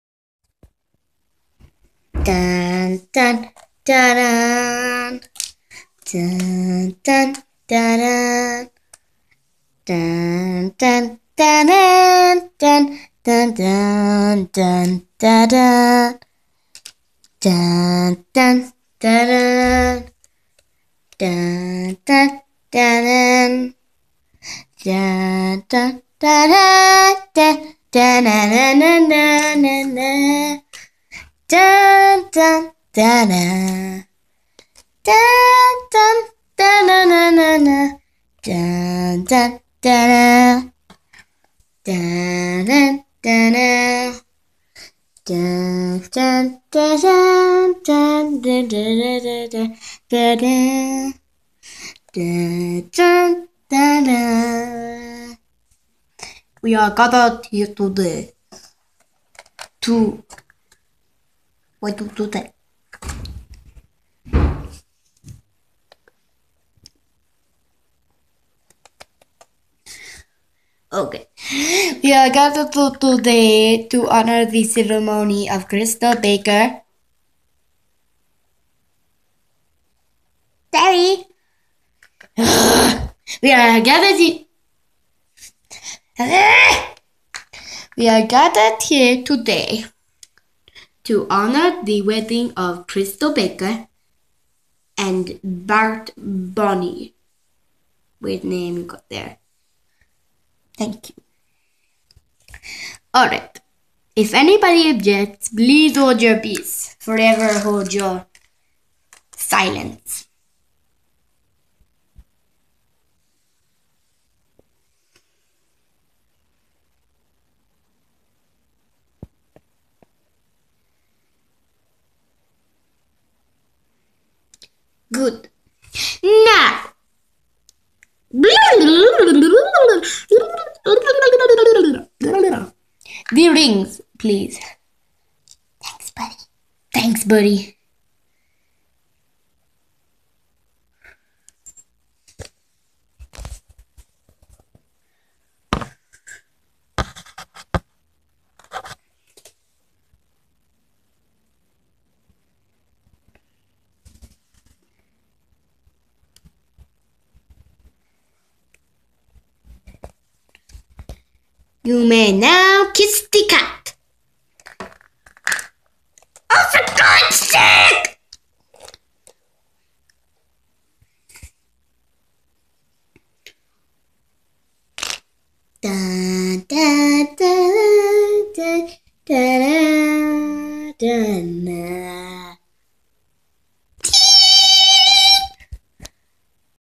Lps wedding
this is the new lps wedding as the first one lost sound half  way through I don't know why though